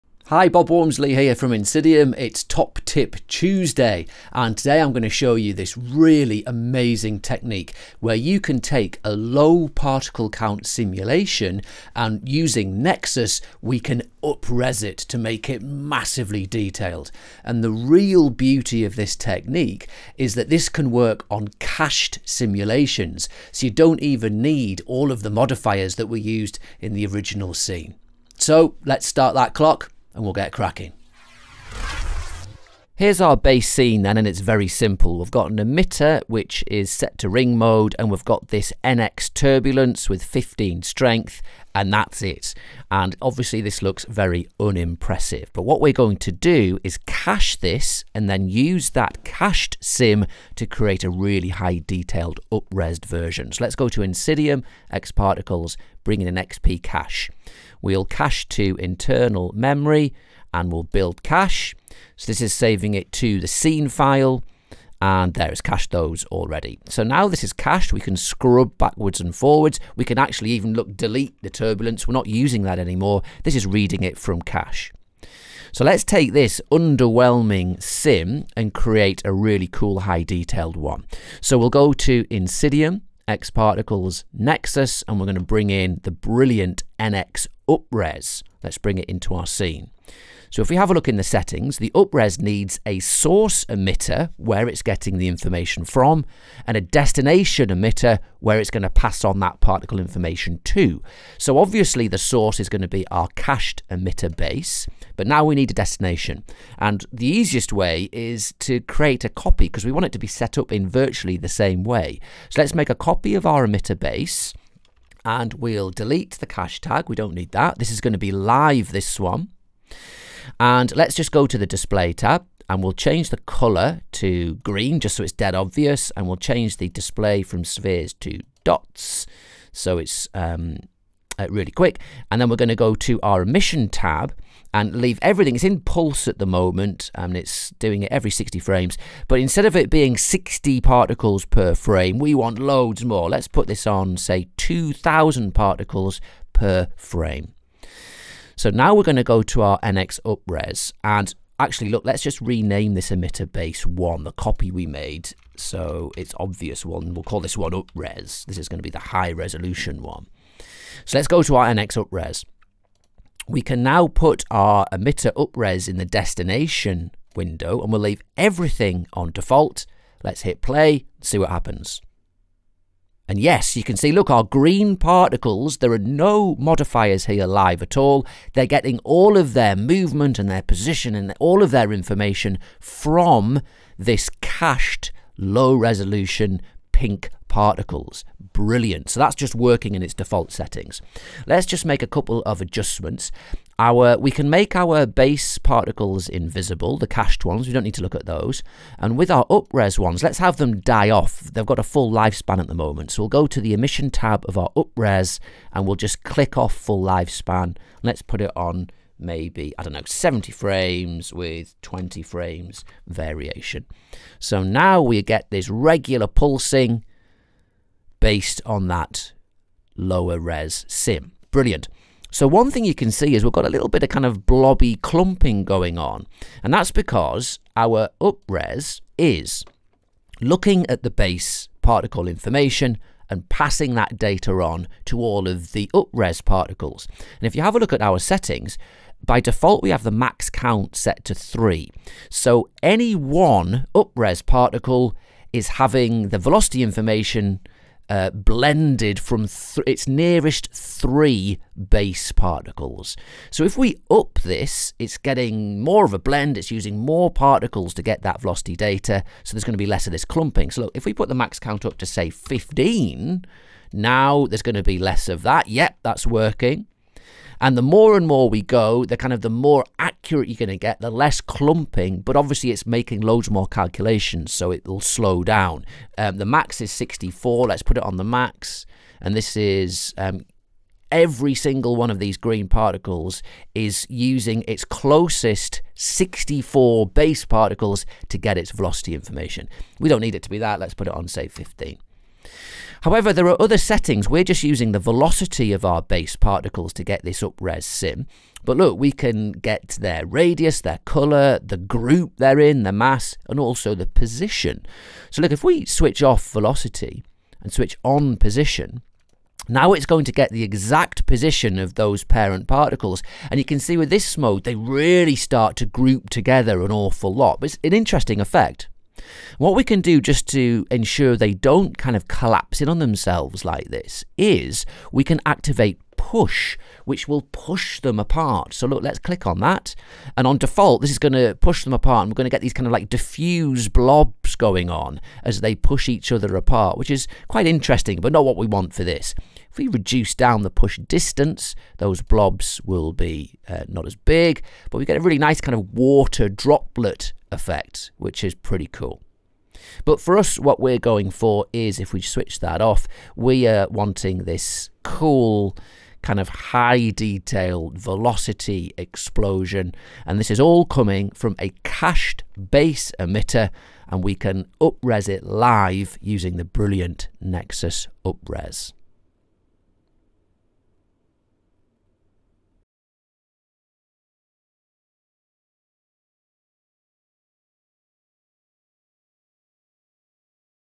Top Tip Tuesday! - nxUpres
In today's Top Tip, we're going to create a low particle-count simulation.

We'll then utilize nxUpres to create a high-detail particle simulation, based on the cached scene.

Then, we'll explore the nxUpres settings for different looks and effects.